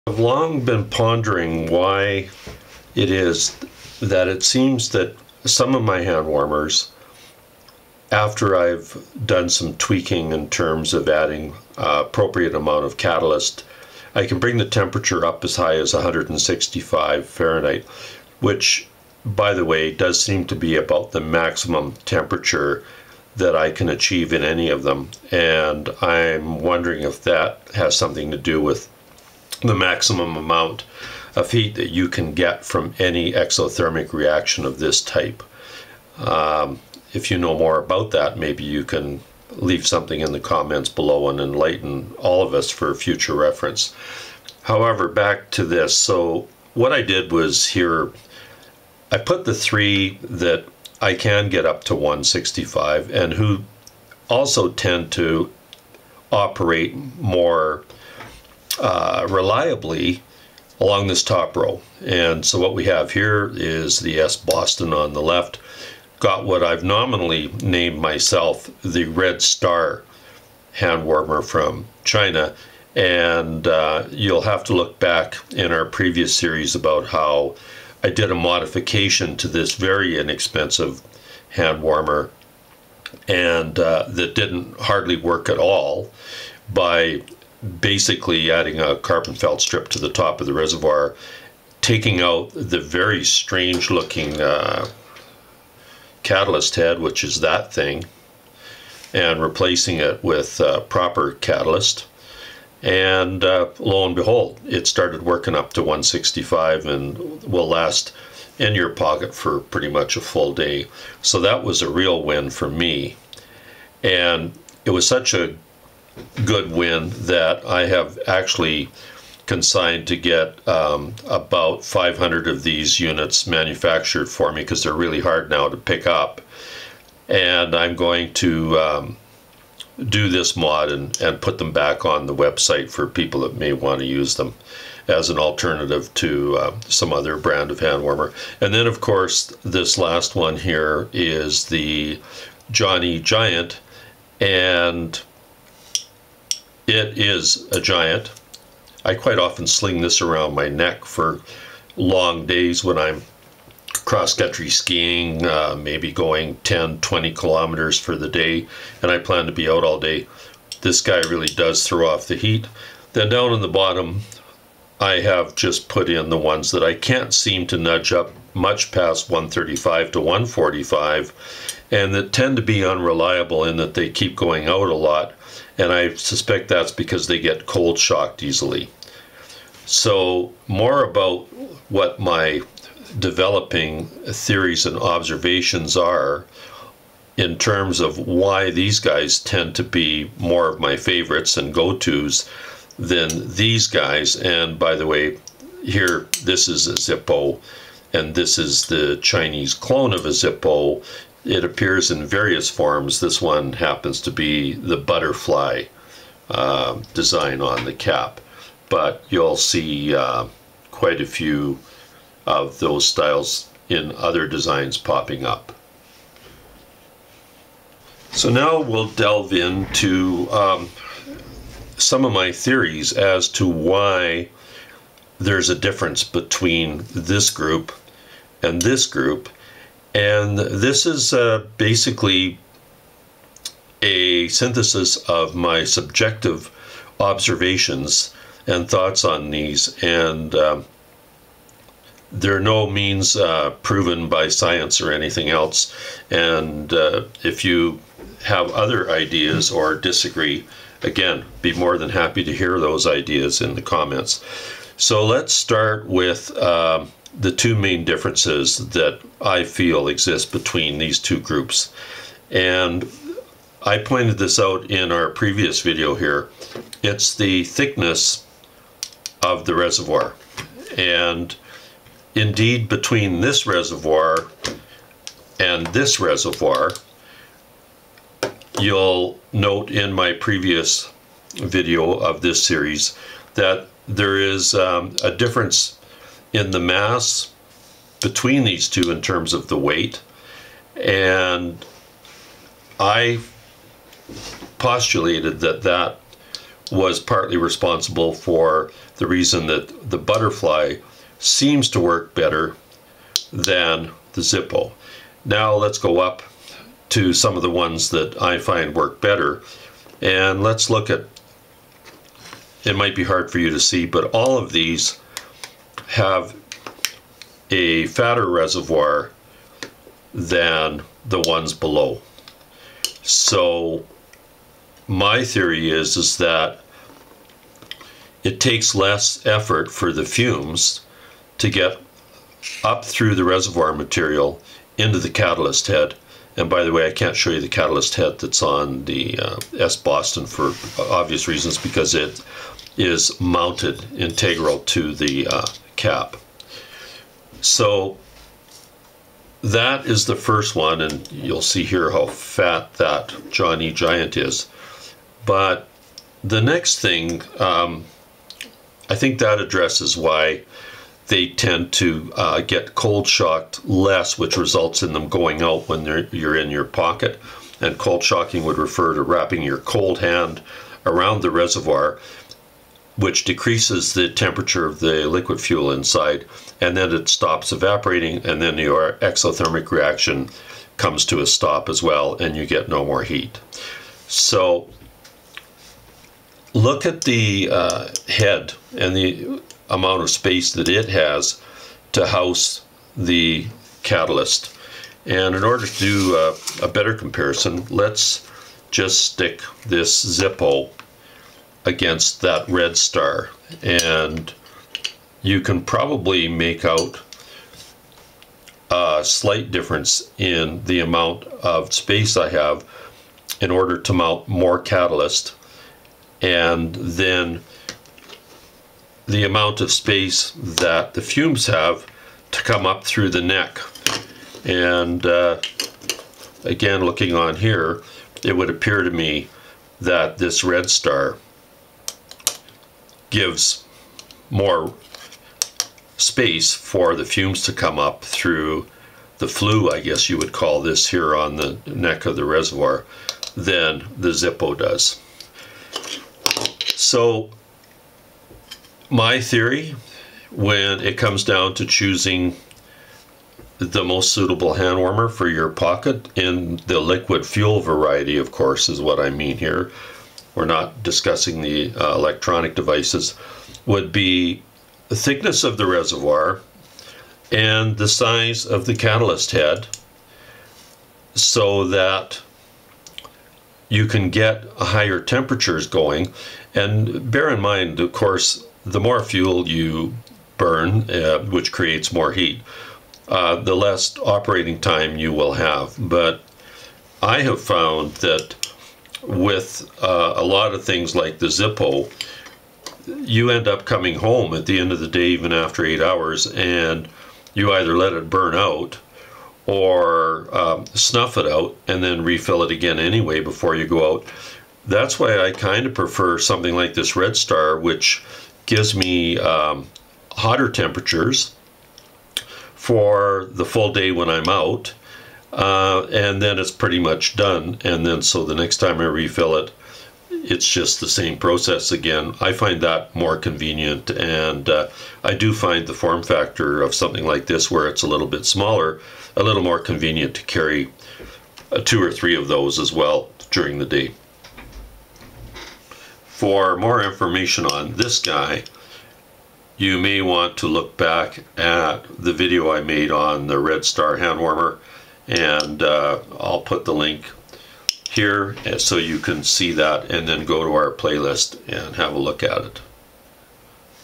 Handwarmers S07E03: What to watch for when buying a hand warmer
Why do some of my hand warmers seem to work reliably, while others are hard to activate and keep running when in my pocket?

Why can I get some of them up to 165F, while no amount of extra catalyst in the head won't get others anywhere north of 145F?

For years I have struggled with coming up with a definitive answer of how to improve the most ubiquitous of all the hand warmer units (think, “what am I going to probably buy from Amazon as I contemplate my cold hands I had on my last outdoor excursion”).

While I have not been able to come up with a hack that will improve the engineering of these units, I do have some personal observations that have given rise to my own subjective theories on this topic.

Hand warmer models shown in this presentation (top left to bottom right): S-Boston, Red Star, Jon-e Giant, Butterfly, Zippo. Note: The S-Boston is no longer in production.

00:13 Units that can go to 165F illustrated
01:15 The Red Star hand warmer – advantages
02:20      Jon-e Giant
02:45      Units I cannot get to heat above 145F
03:45      Theories: Thermal Flywheel effect and catalyst
05:35 constraints to move fumes from reservoir to catalyst head
06:00 'Cold Shock' defined
06:30 Catalyst heads compared
06:45 Catalyst heads, Zippo compared to Red Star
07:00 reservoir stacks compared to illustrate effect on fumes getting into catalyst (Zippo hand warmer and Red Star compared)
07:30 Considerations for the most suitable hand warmer for your pocket?
08:30 Reasons why the Red Star is preferred

Links:
Rebuild parts, order form: